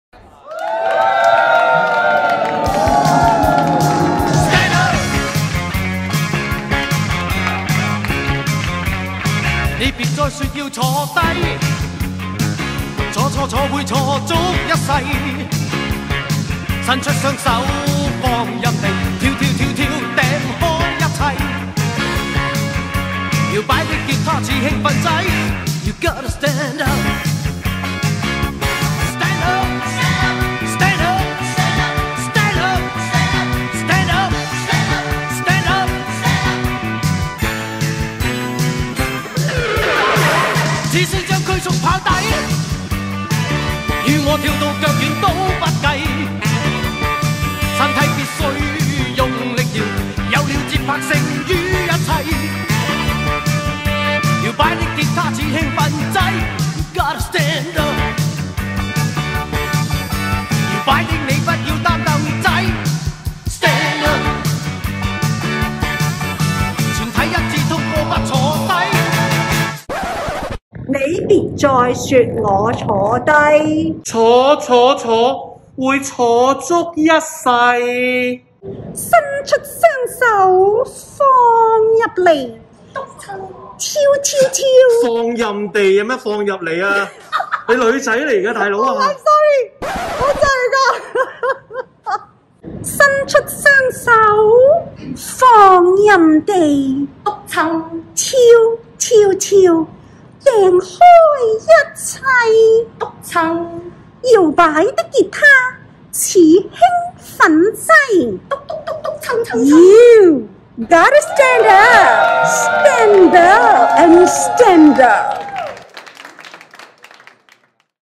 2021 加拿大中文電台 瑪嘉烈公主癌症中心 直播籌款大行活動「舞動全台，跳出個未來」- 燈燈燈凳 YOLO 篇
加拿大中文電台與瑪嘉烈公主癌症中心攜手舉行「抗癌支援計劃」直播籌款大行動 2021
Fairchild Radio and Princess Margaret Cancer Centre Radiothon 2021